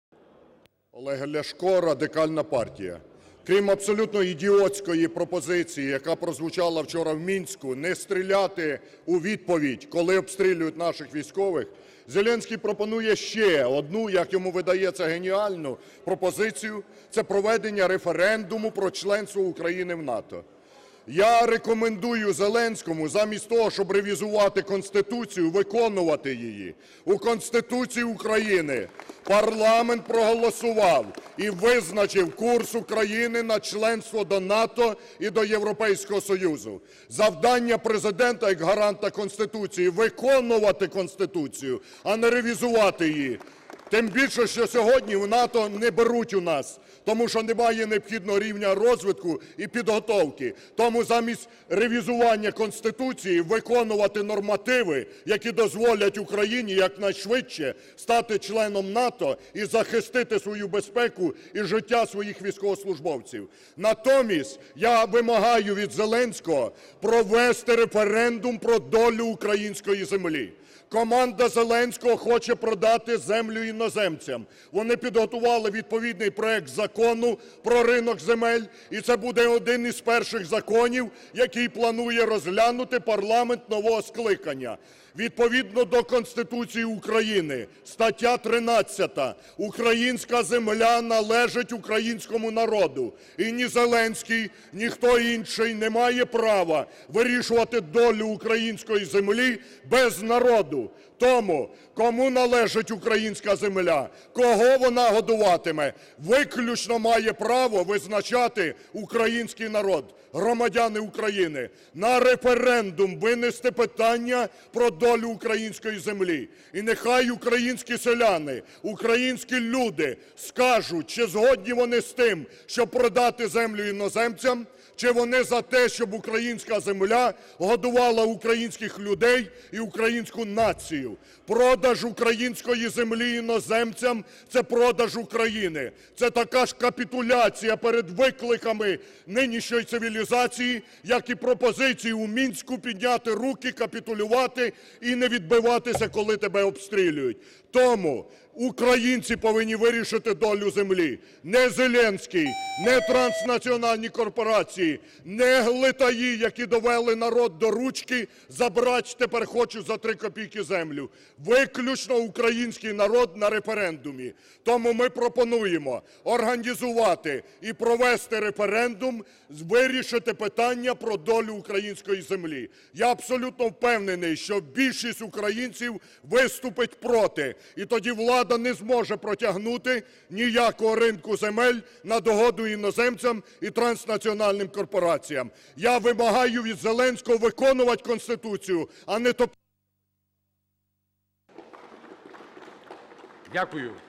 Ляшко: Продаж землі – така ж капітуляція, як і припинення вогню у відповідь
Замість референдуму про членство України в НАТО, Олег Ляшко пропонує Володимиру Зеленському провести референдум щодо продажу землі. Лідер Радикальної партії закликав новообраного президента не переписувати Основний закон, а виконувати його.
«У Конституції України визначено курс України на членство в НАТО та ЄС. Завдання президента, як гаранта Конституції, виконувати її, а не ревізувати. Тим більше, що сьогодні в НАТО Україну не беруть, тому що немає необхідного рівня розвитку і підготовки. Тому замість ревізування Конституції я рекомендую Зеленському виконувати нормативи, які дозволять Україні якнайшвидше стати членом НАТО і захистити свою безпеку та життя своїх військовослужбовців», - підкреслив Ляшко.
Політик наголошує, долю української землі має вирішувати виключно народ України, а не чиновники чи президент.
«Команда Зеленського хоче продати землю іноземцям. Вони підготували необхідний законопроект про ринок земель і це буде один із перших законів, який планує розглянути парламент нового скликання. Відповідно до статті №13 Конституції України, українська земля належить українському народу. І ні Зеленський, ні будь-хто інший не має права вирішувати долю української землі без українського народу. Тому ми вимагаємо винести питання про продаж землі на референдум і нехай українці скажуть – українську землю треба продавати іноземцям чи українська земля має годувати українську націю», - зауважив лідер РПЛ.
Олег Ляшко переконаний, запровадження ринку землі за нинішнього законодавства – це позбавлення українців майбутнього.
«Продаж української землі іноземцям – це продаж України, це така ж капітуляція перед викликами нинішньої цивілізації, як і пропозиції у Мінську підняти руки і здатися, коли тебе обстрілюють. Тому я абсолютно впевнений, що більшість українців на референдумі виступить проти продажу землі і тоді влада не зможе протягнути ніякий законопроект про ринок землі на догоду іноземцям і транснаціональним корпораціям», - підсумував Ляшко.